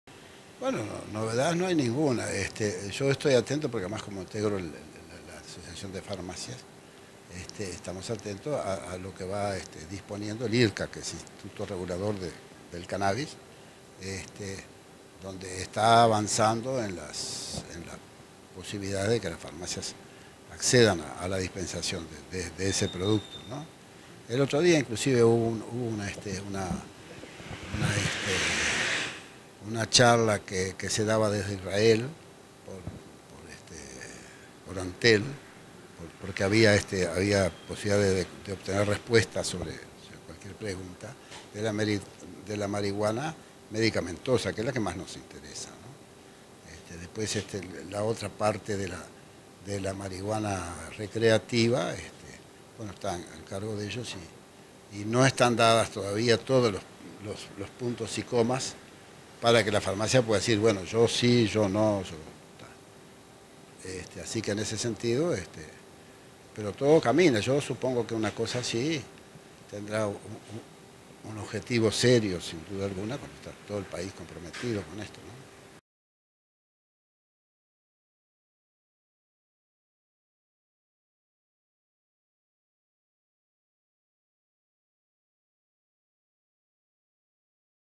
José Federico Díaz - Propietario Farmacia Porto
En cuanto a la actualidad farmacéutica y sobre los avances que ha habido con respecto a la posible venta de cannabis en las farmacias del país Federico diaz expreso lo siguiente.